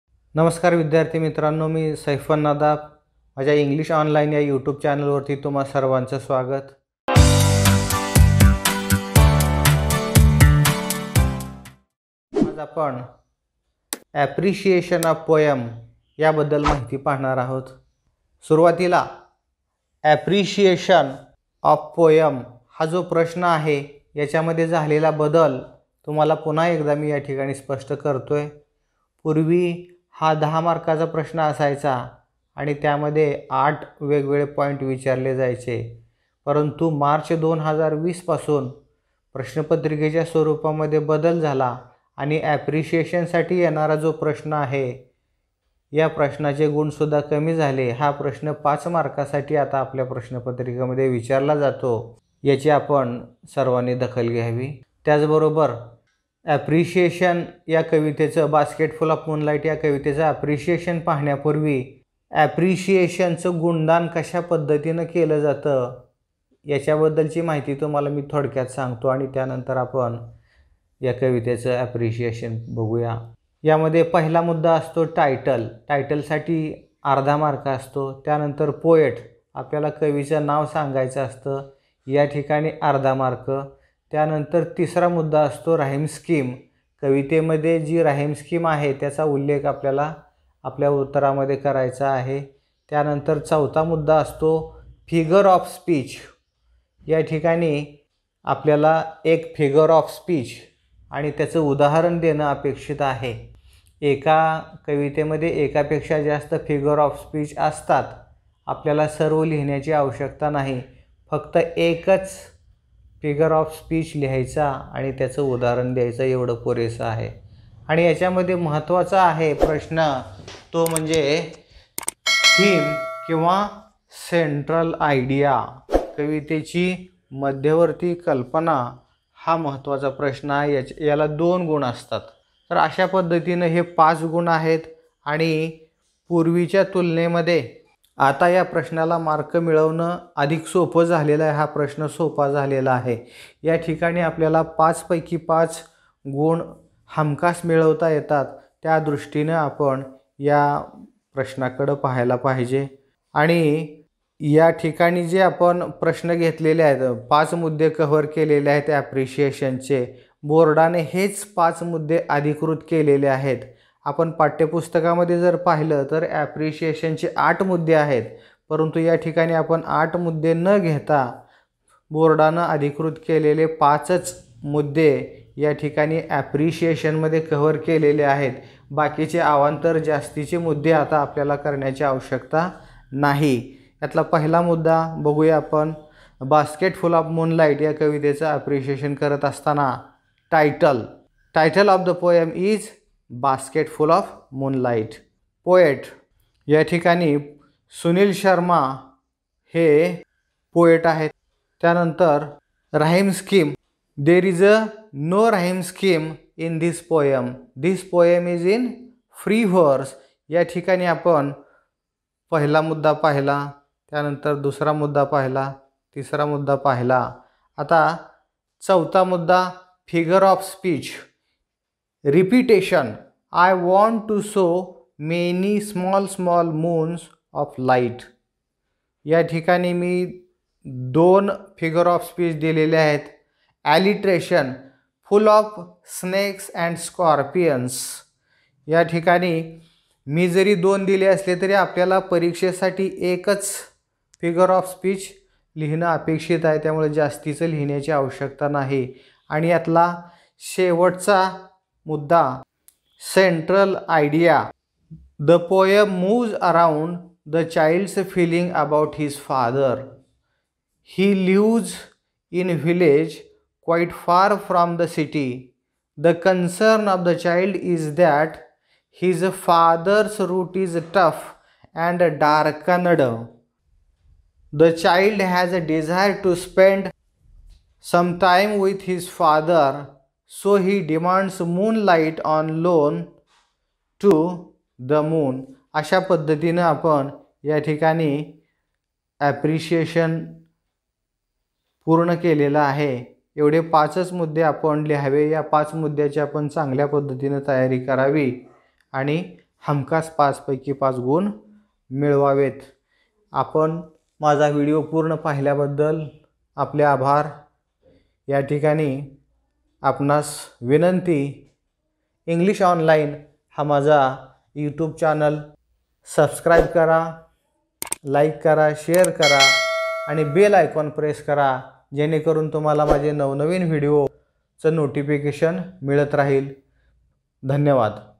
10th std | The Appreciation of poem "1.3 Basketful of Moonlight" .By Saifan Nadaf.
The Appreciation of poem "1.3 Basketful of Moonlight" by Saifan Nadaf. 10th std | English.